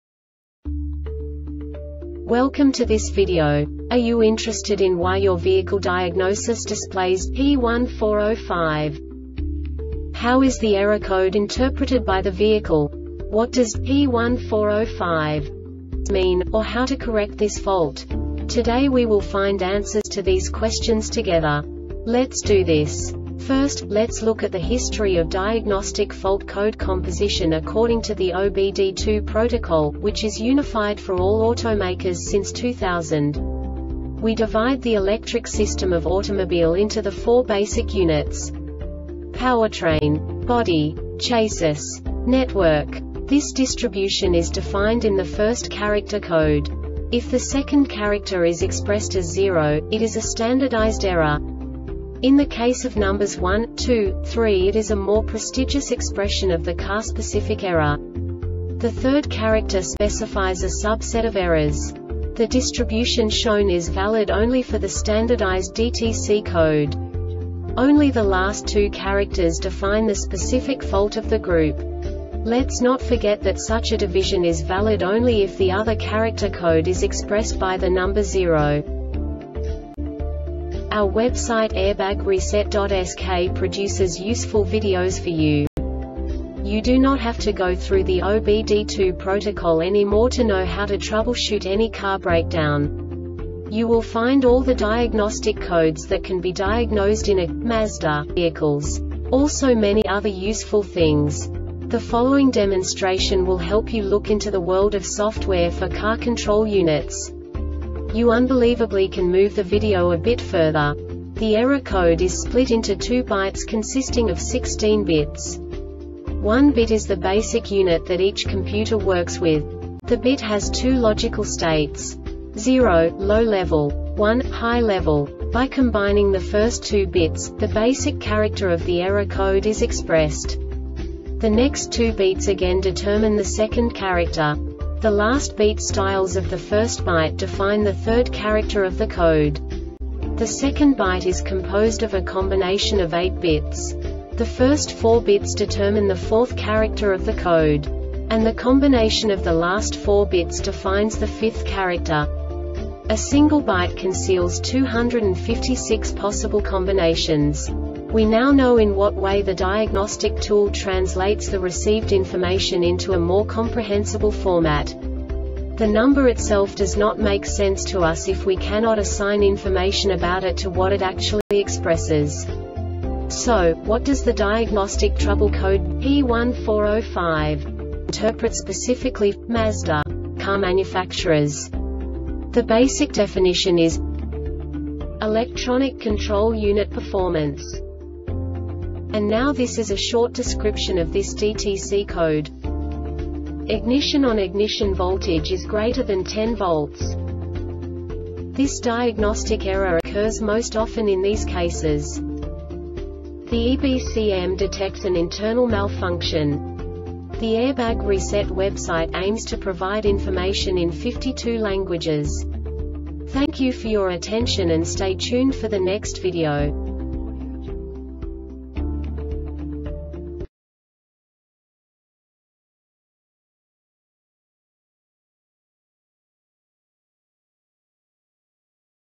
DTC Mazda P1405 Short Explanation
Contents:
0:21 Basic DTC analysis according to OBD2 protocol standard.
1:48 Insight into programming
2:58 A diagnostically understandable description of the Diagnostic Trouble Code
3:05 Basic error definition

Make:
Mazda
Code:
P1405
Definition:
Electronic Control Unit Performance
Description:
Ignition ON.Ignition voltage is greater than 10 volts.